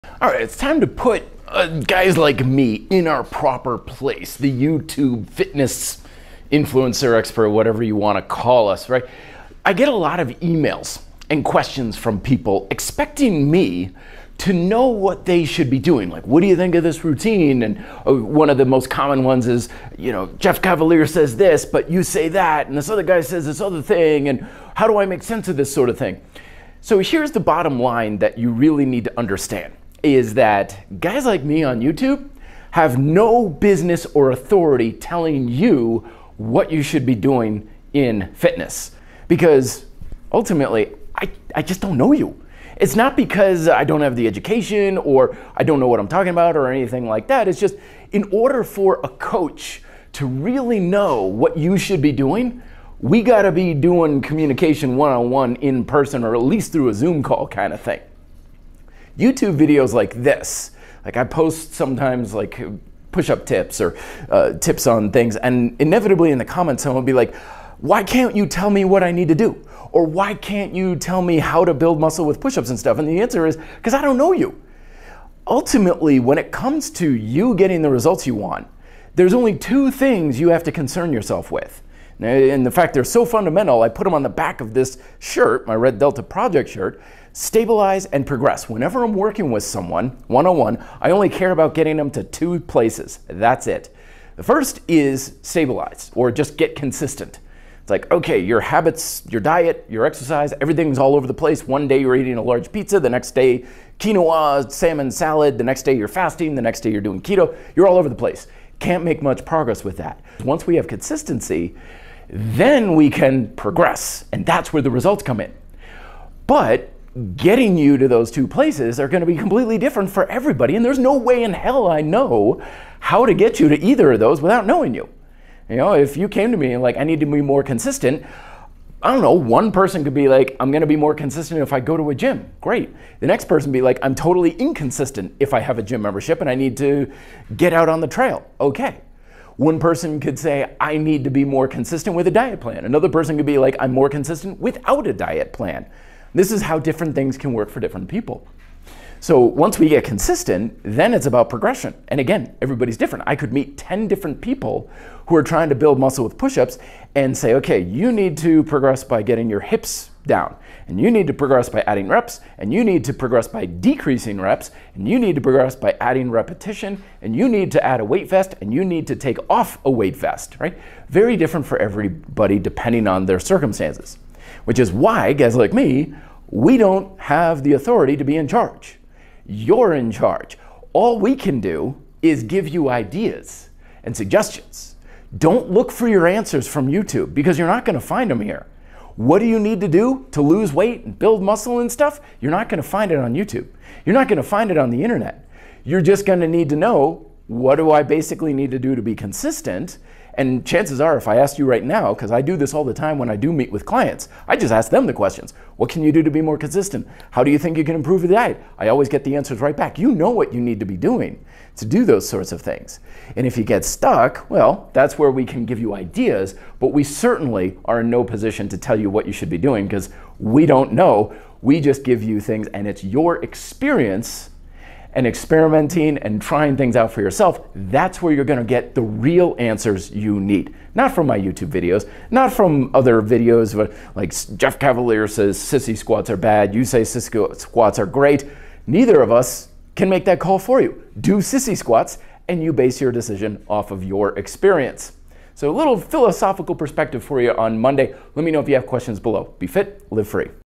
Putting Us Fitness YouTubers In Our Proper Place
We fitness YouTubers put out a ton of daily content on how to diet, exercise, build muscle, lose weight, and maybe even build your own home gym. But for all of that information, none of it can definitively tell you what you personally need to do to get the results you want.

RDP books-
Micro Workouts

Grind Style Calisthenics

Bodyweight Training for Cycling